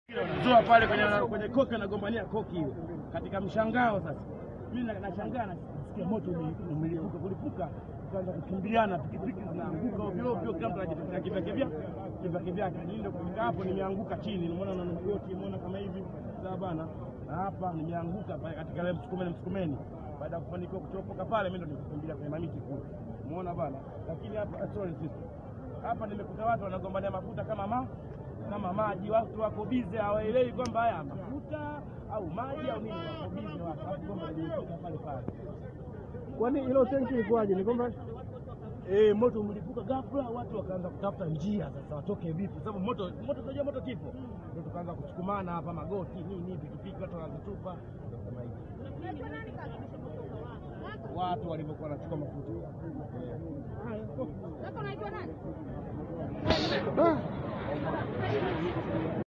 MAJONZI: SHUHUDA WA AJALI YA GARI LA MAFUTA ASIMULIA KILICHOTOKEA.
Shuhuda akizungumzia jinsi ajali ya gari la mafuta ilivyotokea iliyopelekea kuua watu 54 eneo la Msamvu mkoani Morogoro